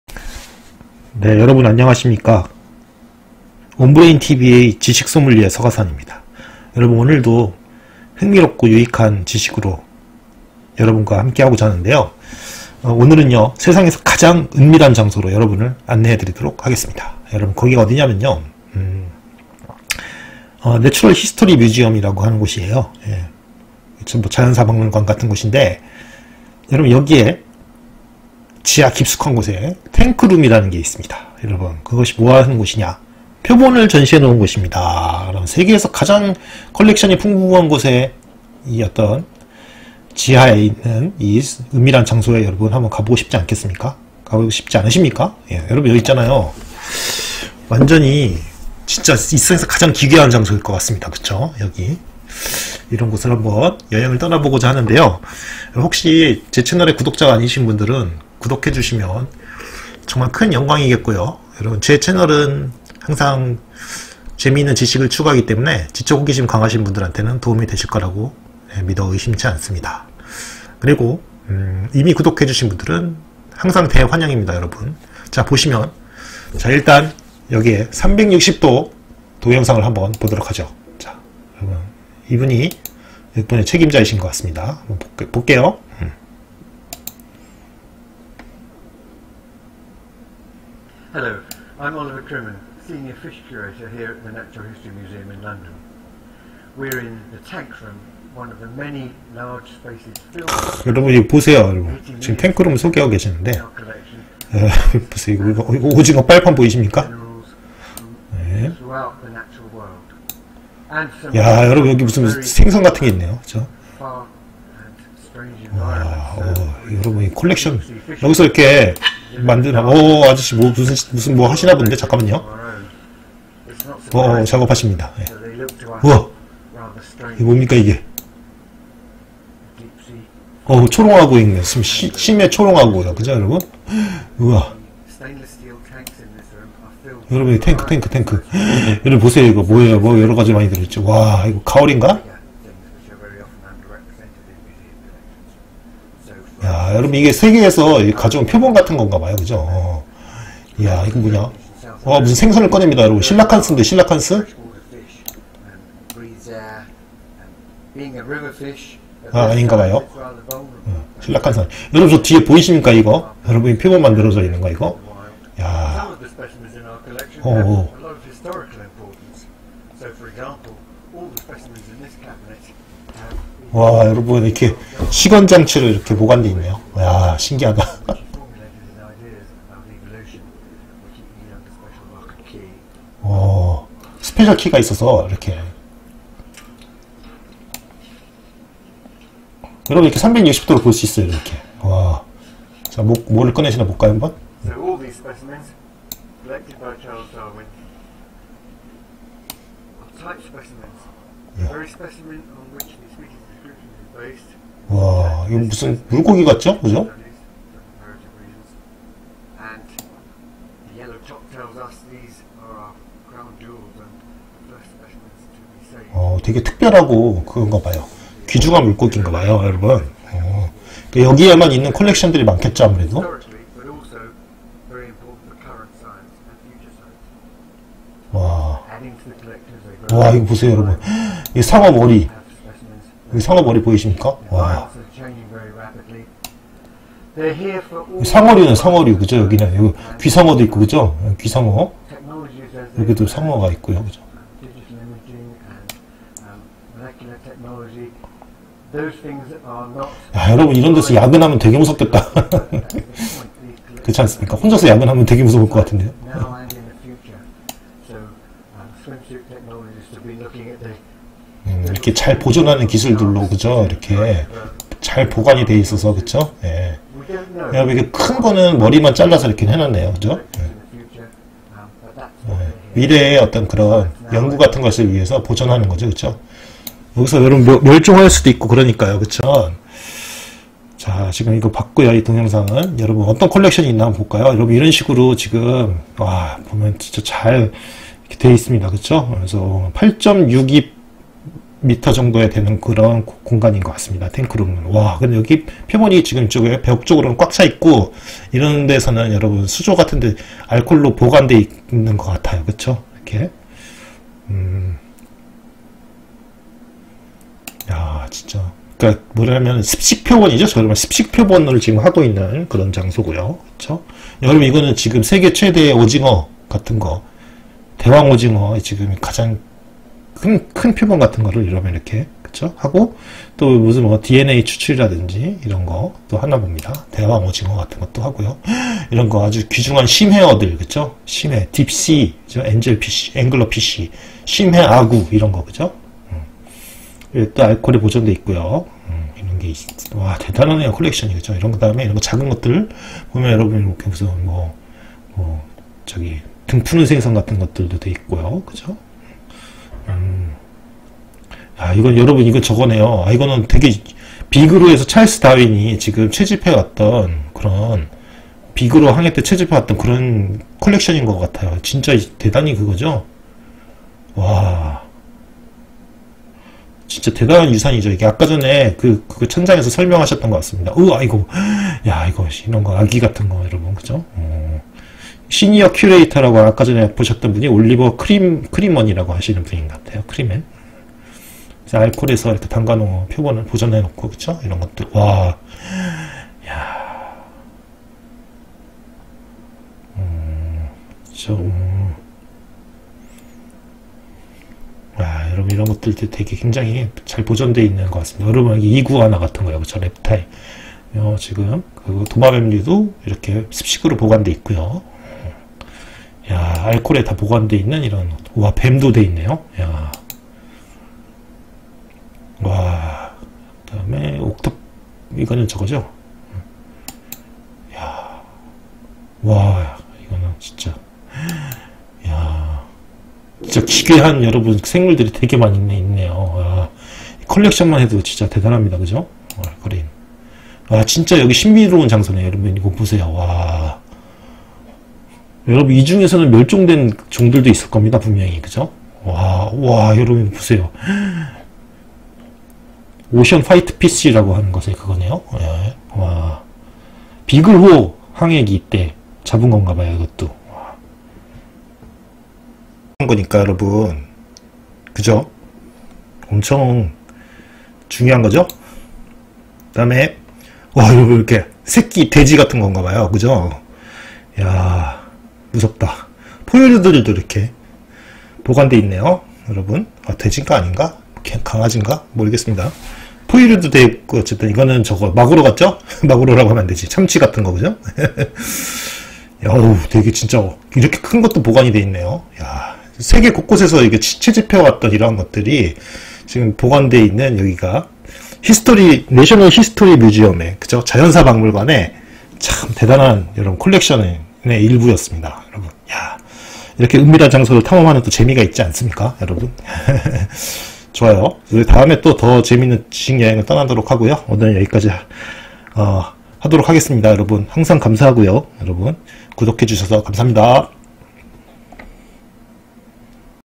세상에서 가장 은밀하고 기묘하며 흥미로운 장소
그곳은 바로 자연사박물관의 탱크룸입니다. 함께 가보시죠^^